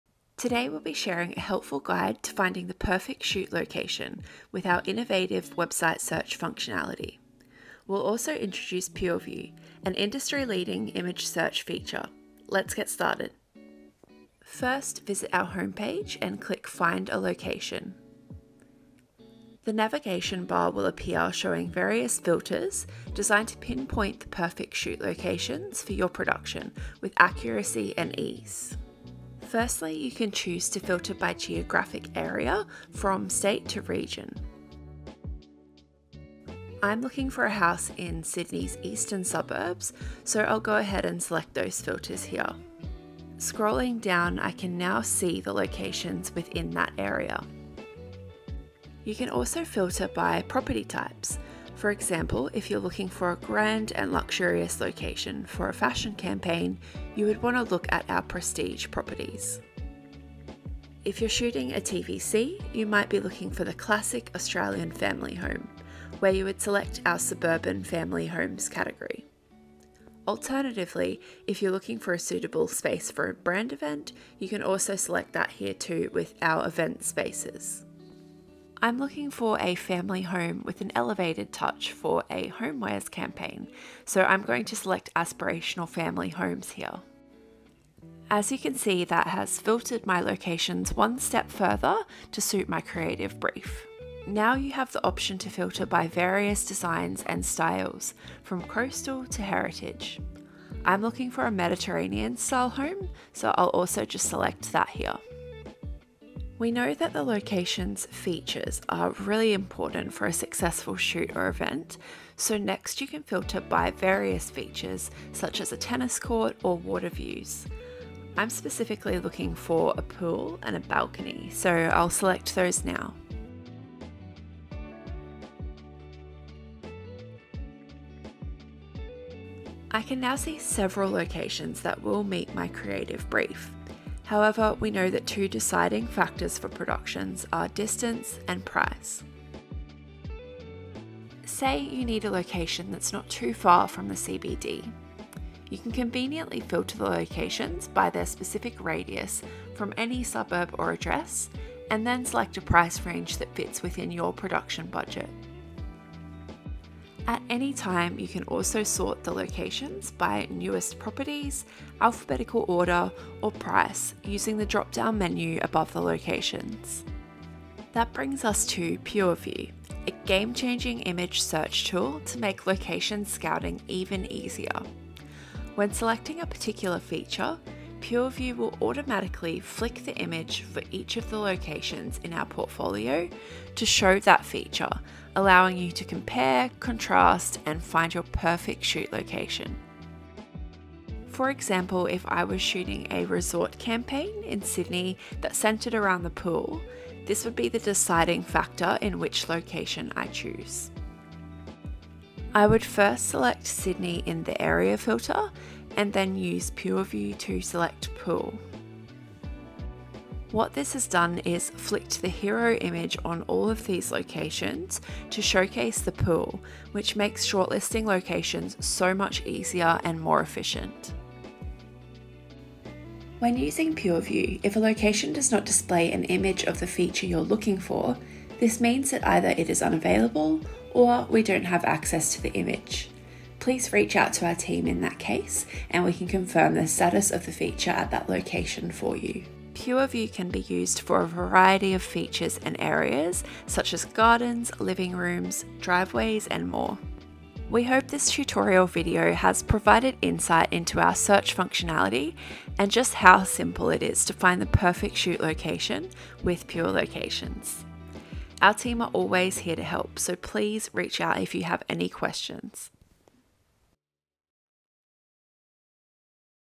Location Scouting Search Tools | Pure Locations Website Tutorial
Finding the perfect location for your brand production has never been easier. At Pure Locations, we are dedicated to simplifying the location scouting process for brand creatives by championing innovation and continuous digital improvements. Whether you’re searching for a luxurious mansion, cozy family home or a sensational brand event location, our innovative website search functionality is designed to serve up the perfect locations with precision and ease.

00:00 Introduction
00:14 Navigating our location search filters
02:47 Introducing PureView